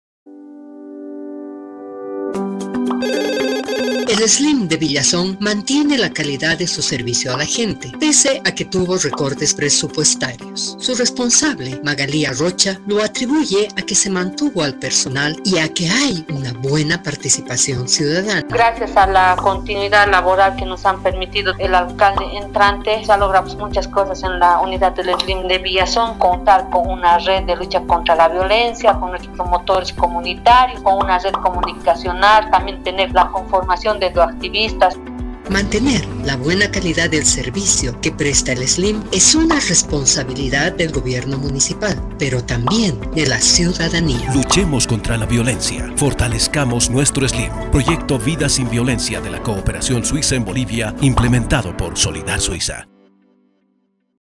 Trabajo SLIM Villazón
¡Los SLIM siguen trabajando!

Mira el video y conoce un poco de como se trabaja en el Municipio de Villazón en la lucha contra la violencia.